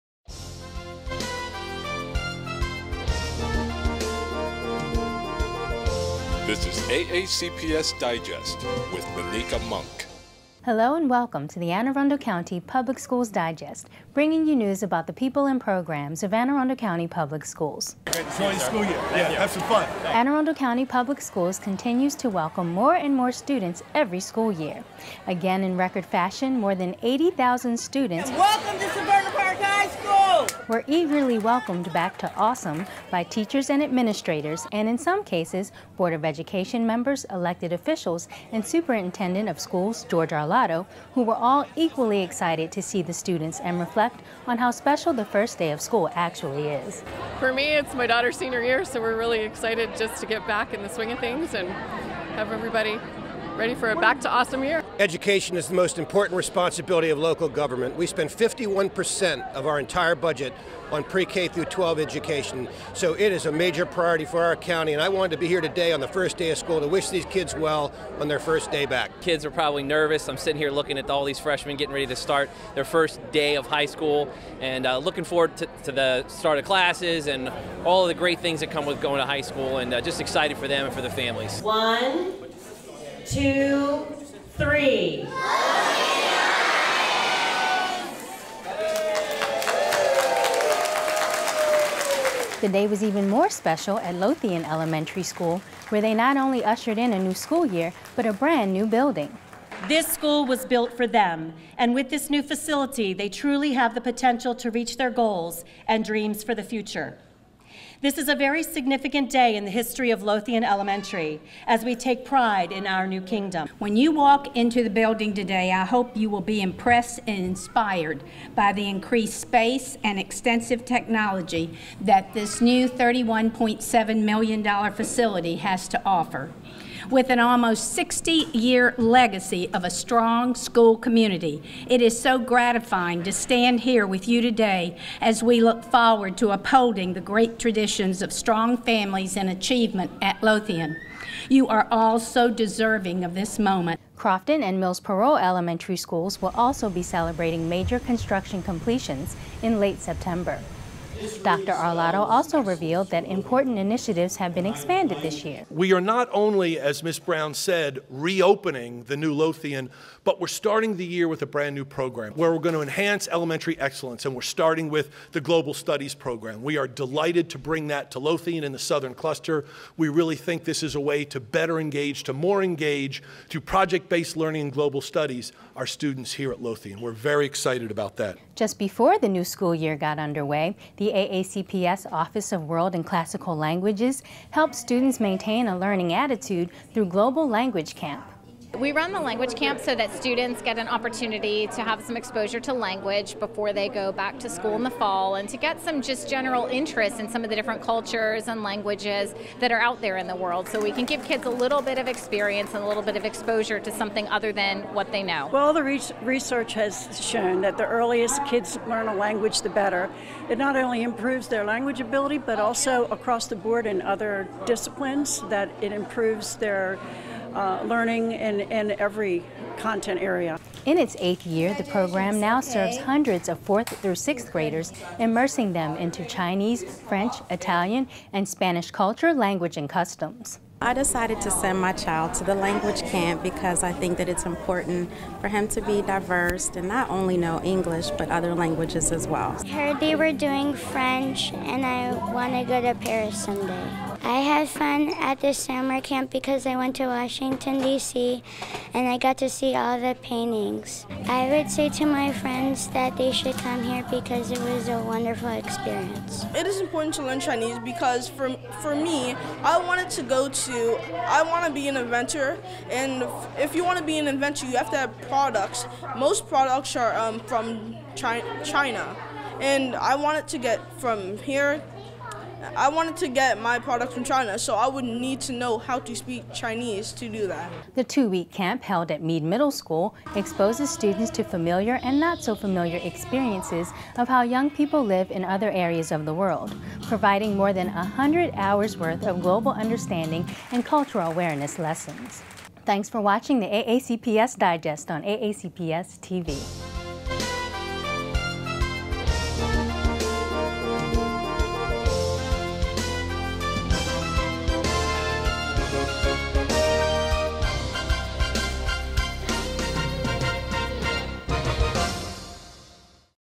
AACPS-TV News Digest - September 2015
AACPS-TV News Digest with Maneka Monk.
An Anne Arundel County Public Schools news program.

• World & Classical Languages' Global Language Camp